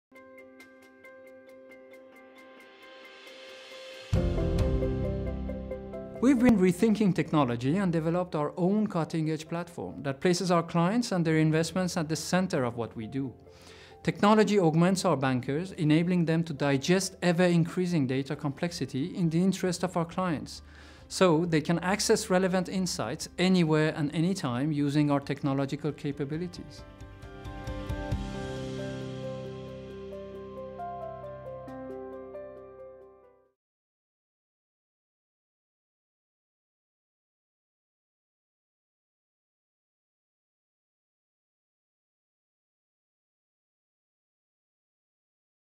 Rethinking Technology
To most, private banking and cutting-edge technology do not seem to be obvious associates. Yet, in today's data-driven world, not only is technology useful to wealth management. We believe that the right tools are vital if we are to design the most successful investment strategies for our clients.

For us, this means employing technology that enables us to exploit data effectively. But this simple-sounding statement belies its difficulty. For as data grows more powerful, it also grows more complex.

Our clients told us that our platform had become a valuable part of our offering because it offered them a crystal-clear picture of the composition and performance of their portfolios whenever they wanted it. Today, we license our technology to many other institutions who want to offer that same value to their clients.

The decision to design our own platform was a turning point in the history of our bank. By continually rethinking technology, we can now conduct highly sophisticated manipulations of the large volumes of data at our fingertips. And through such analysis, combined with over 220 years of experience, we gain valuable insights that enable us to offer our clients the ideal combination of personal consulting and technology-supported services.